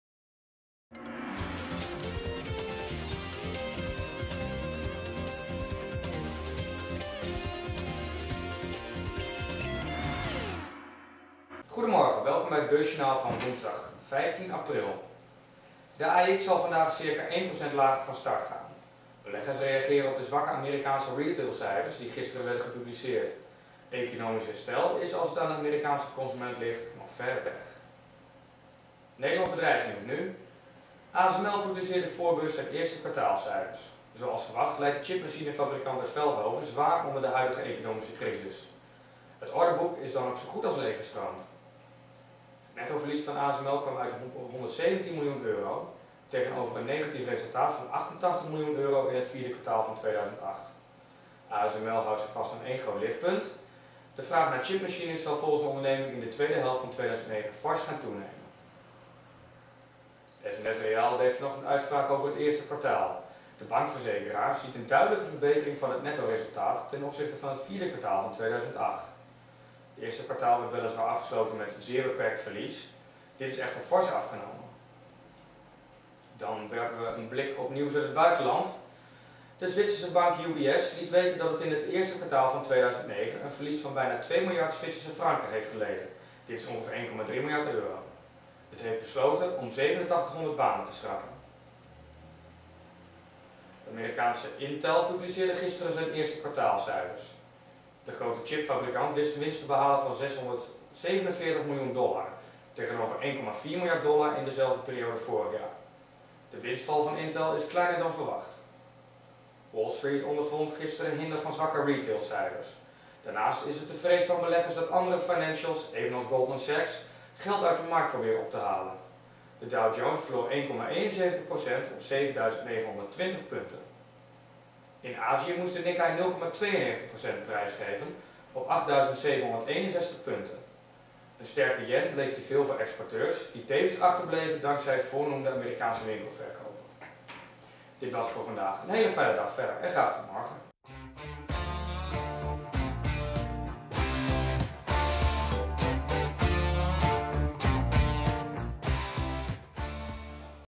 Damrak opent lager na zwakke economische data
Amerikaanse winkelverkopen vallen tegen, ASML, SNS Reaal, Intel en UBS deden de boeken open.